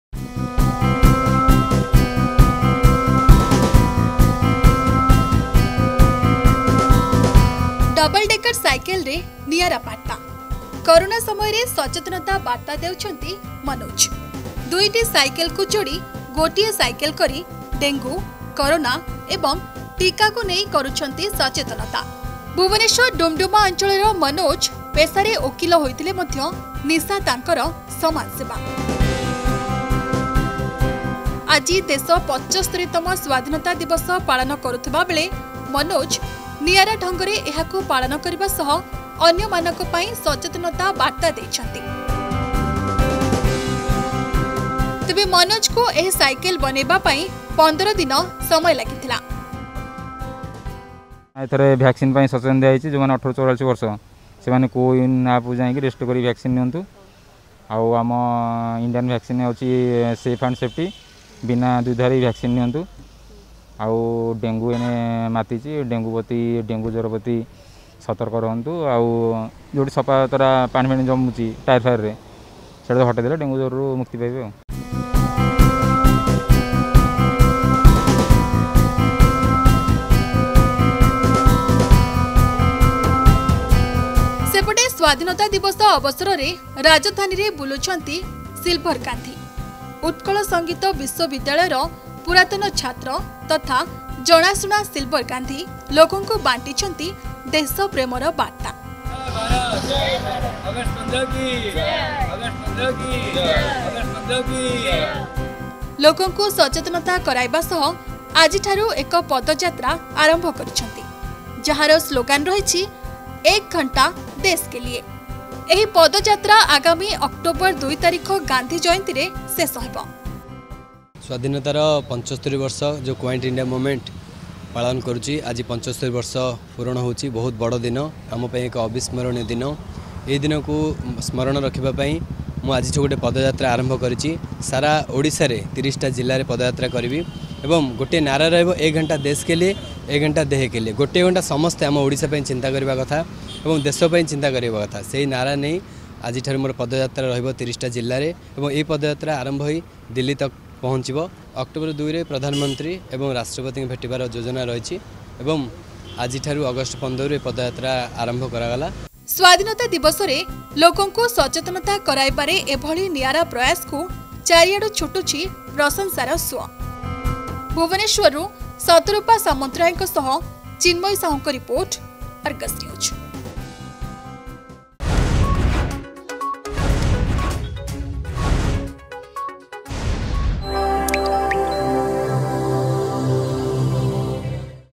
ଏକ ଘଣ୍ଟା ଦେଶ କେ ଲିଏ
ଡବଲ ଡେକର ସାଇକେଲରେ ନିଆରା ବର୍ତ୍ତା । କରୋନା ସମୟରେ ସଚେତନତା ବାର୍ତ୍ତା ଦେଉଛନ୍ତି ମନୋଜ । ଦୁଇଟି ସାଇକେଲକୁ ଯୋଡି ଗୋଟିଏ ସାଇକେଲ କରି ଡେଙ୍ଗୁ,କରୋନା, ଏବଂ ଟୀକାକୁ ନେଇ କରୁଛନ୍ତି ସଚେତନତା ।  ସେପଟେ ସ୍ବାଧୀନତା ଦିବସ ଅବସରରେ ରାଜଧାନୀରେ ବୁଲିଛନ୍ତି ସିଲଭର ଗାନ୍ଧୀ ।  ଉତ୍କଳ ସଙ୍ଗୀତ ବିଶ୍ୱ ବିଦ୍ୟାଳୟର ପୁରାତନ ଛାତ୍ର ତଥା ଜଣାଶୁଣା ସିଲଭର ଗାନ୍ଧୀ ଲୋକଙ୍କୁ ବାଣ୍ଟିଛନ୍ତି ଦେଶ ପ୍ରେମର ବାର୍ତା । ଲୋକଙ୍କୁ ସଚେତନ କରାଇବା ସହ ଆଜି ଠାରୁ ଏକ ପଦ ଯାତ୍ରା ଆରମ୍ଭ କରିଛନ୍ତି । ଯାହାର ସ୍ଲୋଗାନ ରହିଛି  "ଏକ ଘଣ୍ଟା ଦେଶ କେ ଲିଏ ।